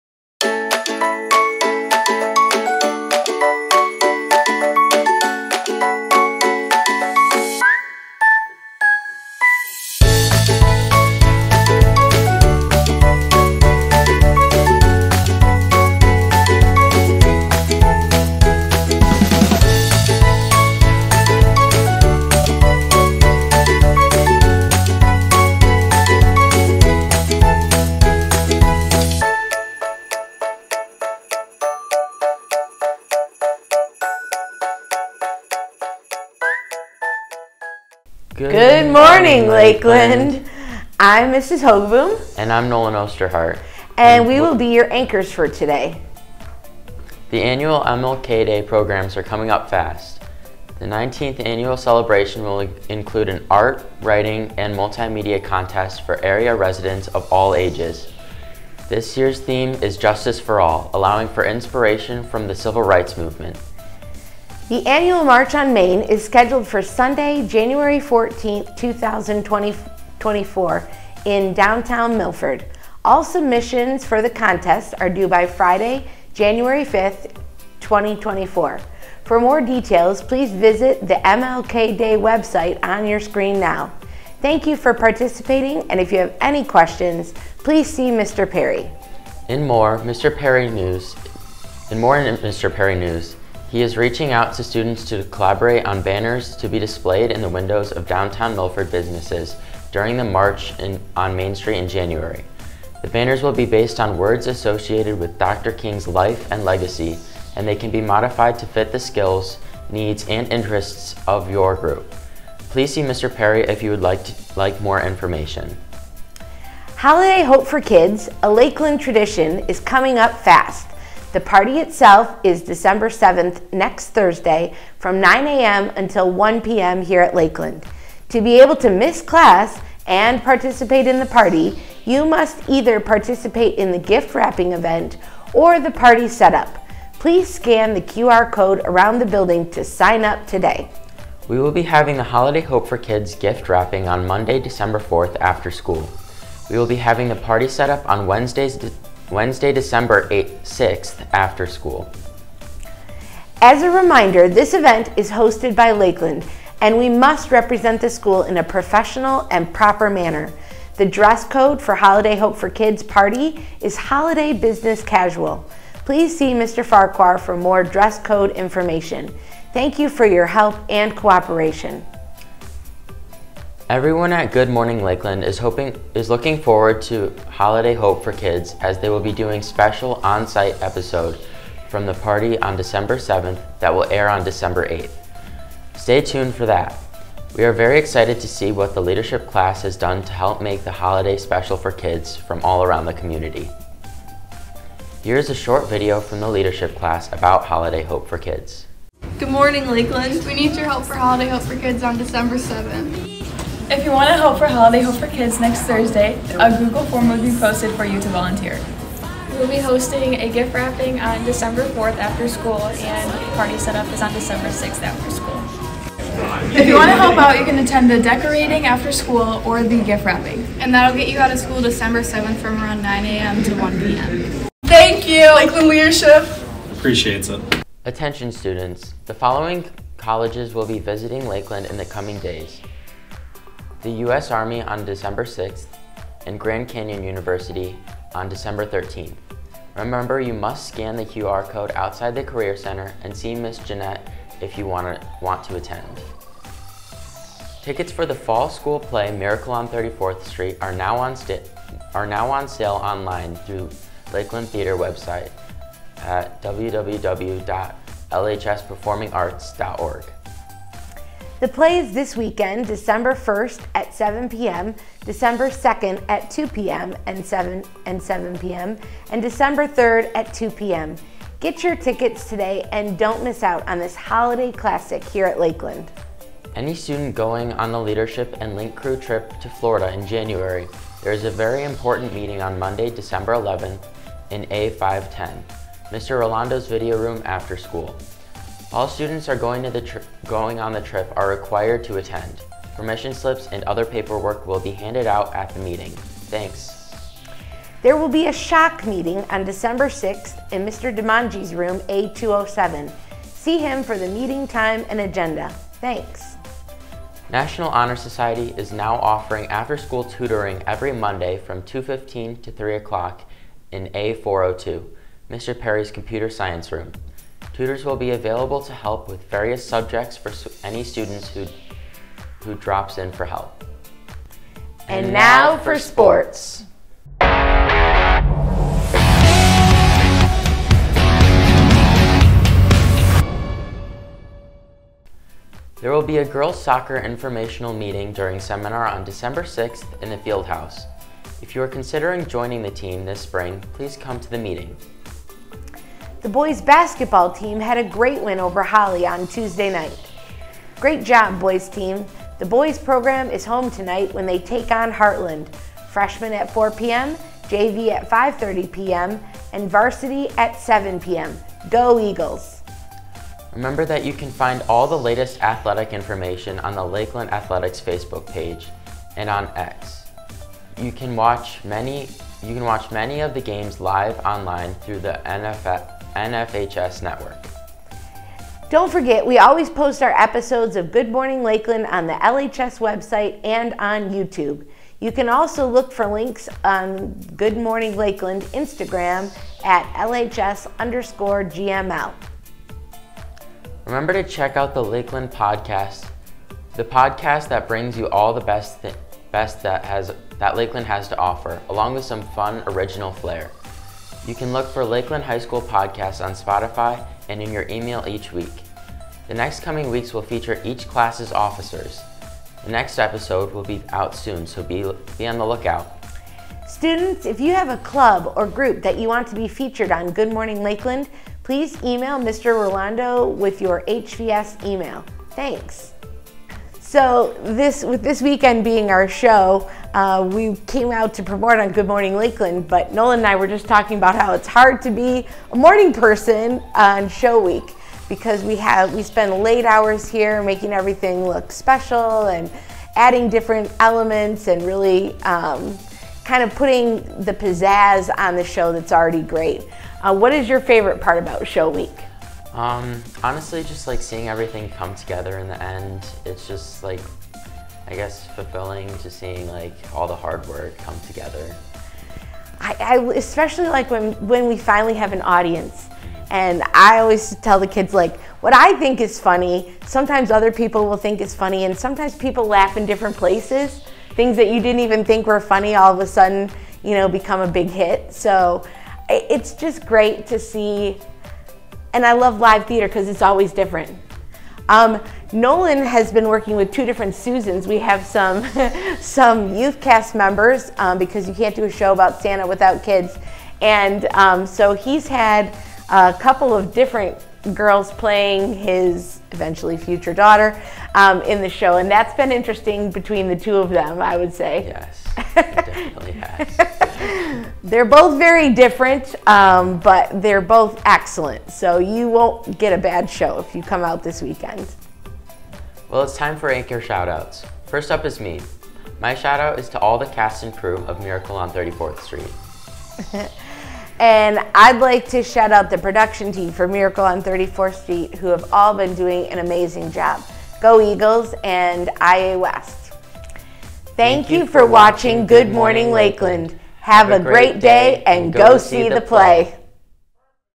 Good Morning Lakeland Friday 12/1/2023 Episode #13
Welcome to GML today! Today we have Nolan and Mrs. Hogaboom promoting the school play Miracle on 34th Street and all the other great announcements. Enjoy!!